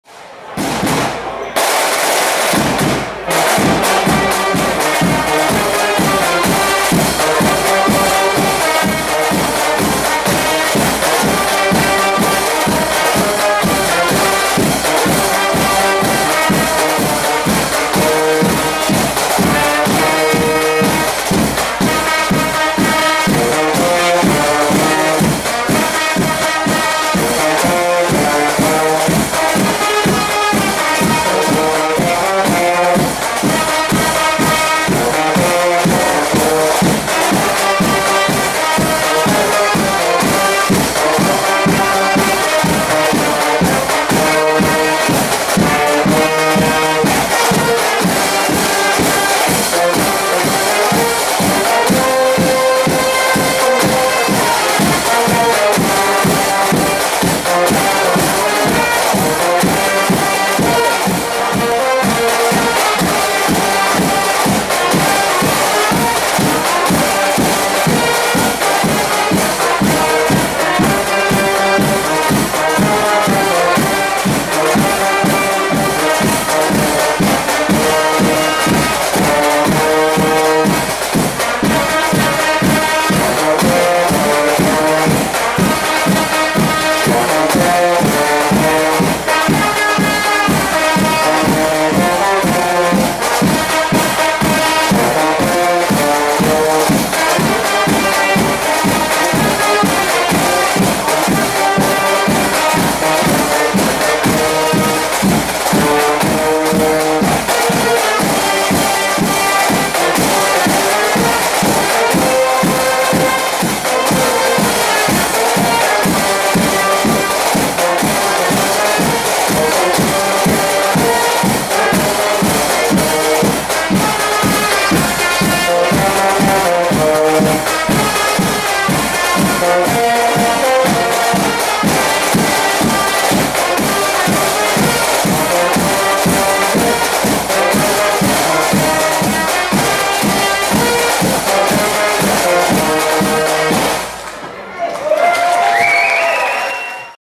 FFZ des TSV Pansdorf, Fröhliche Aalwoche Haffkrug 2019
Der Fanfarenzug des TSV Pansdorf während der Aalwoche 2019 in Haffkrug.